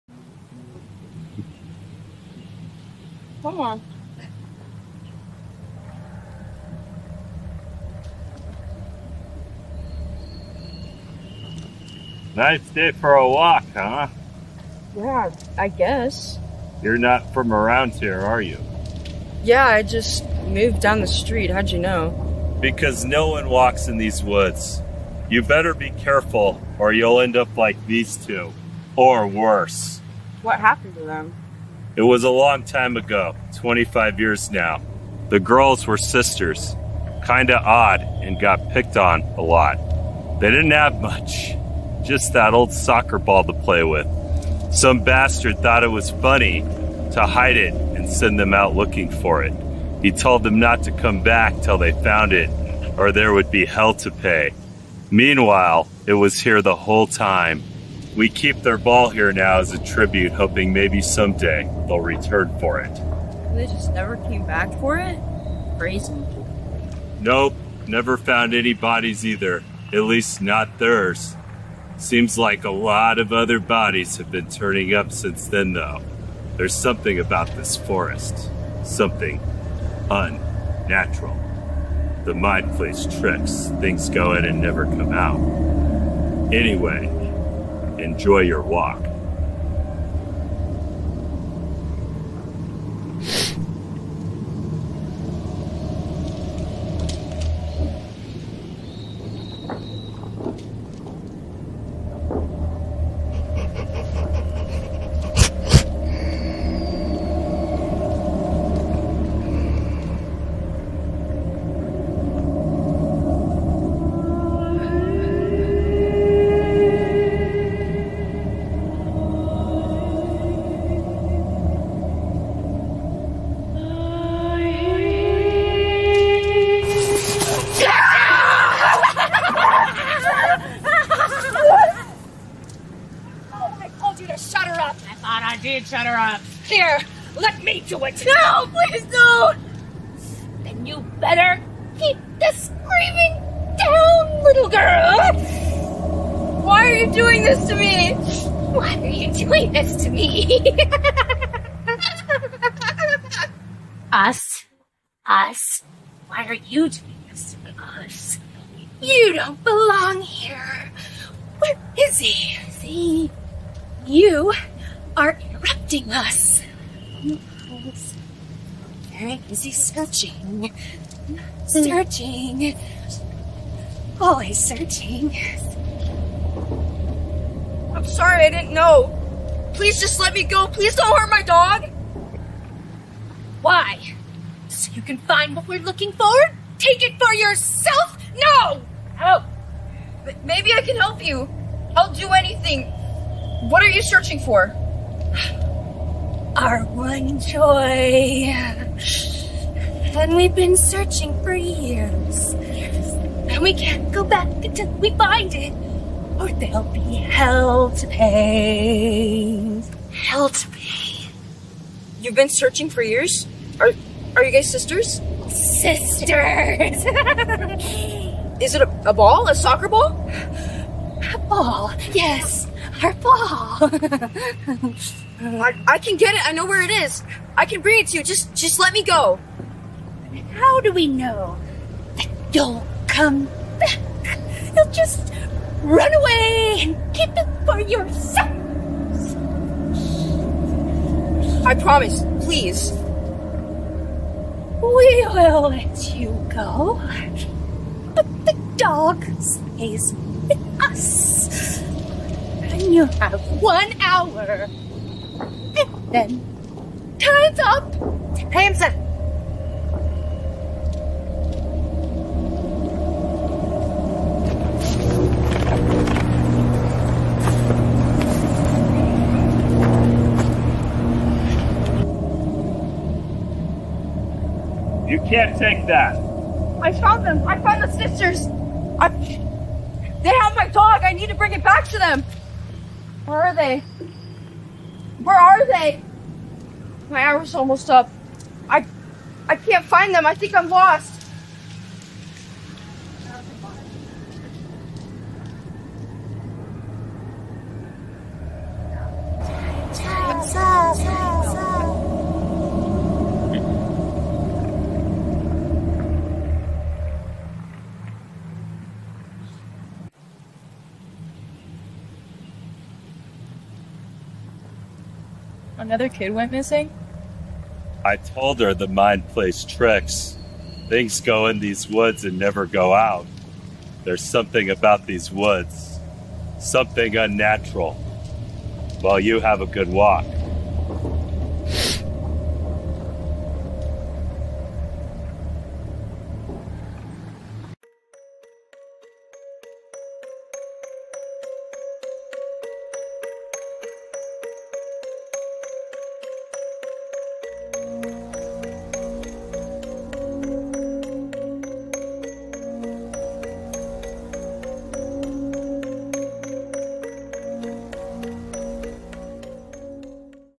Forest For the Trees
Team Name: Ghost Cat Productions
Team Leader: Laura Landry

Synopsis: In a small town, a new resident sets out for a walk with her dog only to find out that people seem to go missing in the local woods. Things go in and never come out, there's something about those woods, something... unnatural.